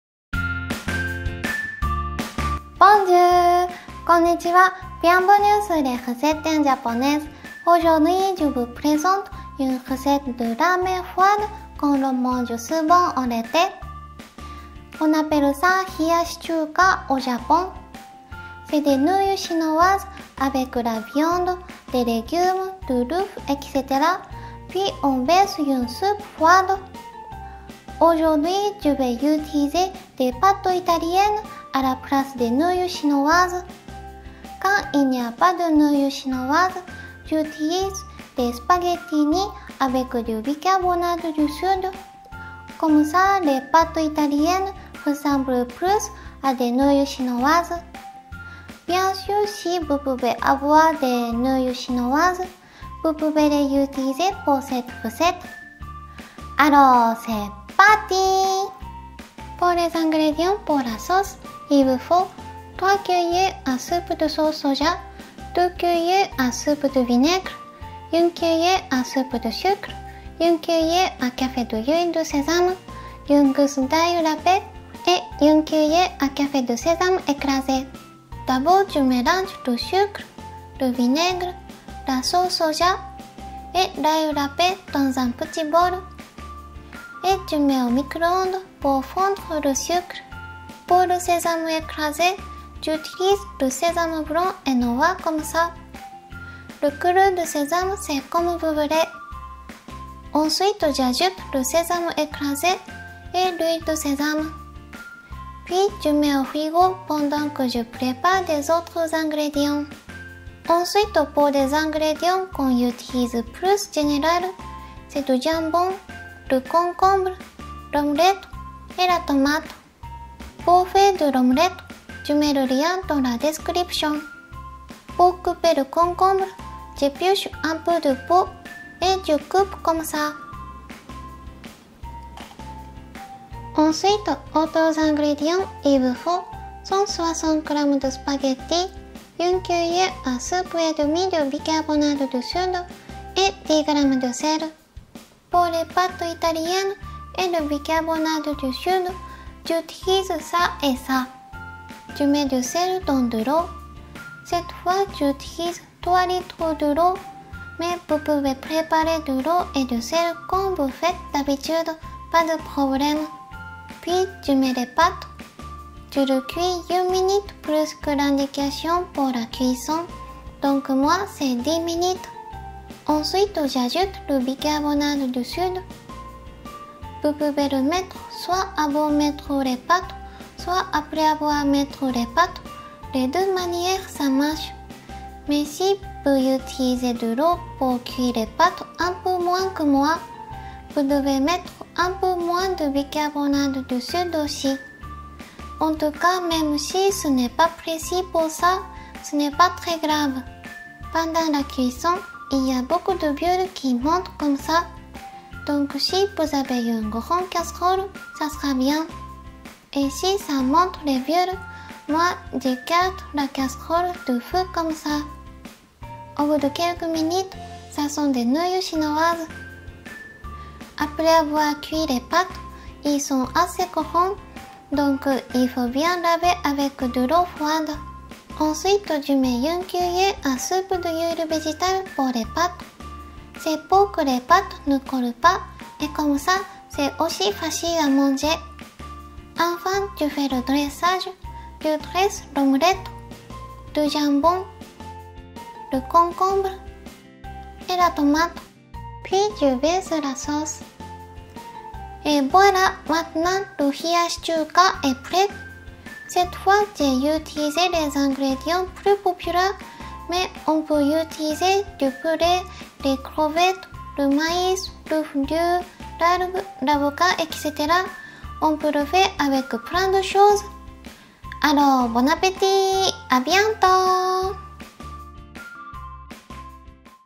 Recette Hiyashi chuka Ramen froid | Les Recettes d'une Japonaise | Cuisine Japon
Recette Hiyashi chuka (Ramen froid)

Ingrédients pour 2 personnes:

・3 c à s de sauce soja
・2 c à s de vinaigre
・1 c à s de sucre
・1 c à c d'huile de sésame
・1 gousse d'ail râpé
・1 c à c de sésame écrasé

Autre ingrédients:
・les tranche de jambon
・la tomate

・160 g de spaghetti
・1 c à s et demi de bicarbonate de soude
・1 c à s d'huile végétale

① Pour faire la sauce: Mélanger la sauce soja, le vinaigre, le sucre
et l'ail râpé puis chauffer un peu au micro-onde (pour fondre le sucre).
② Ajouter le sésame écrasé et l'huile de sésame puis mettre au frigo.
③ Préparer les autres ingrédients: Couper du jambon, le concombre, la tomate et faire de l'omelette.
④ Pour cuire des pâtes: Saler de l'eau et cuire des pâtes 1 minute de plus que l'indication ( mettre le bicarbonate de soude aussi).
⑤ Laver bien les pâtes avec de l'eau froid.
⑥ Verser de l'huile végétale dans les pâtes et bien mélanger.
⑦ Dresser les pâtes, du jambon, l'omelette, le concombre et la tomate puis verser la sauce.

A bientôt sur les recettes d'une Japonaise ♡(*'ω'*)ﾉ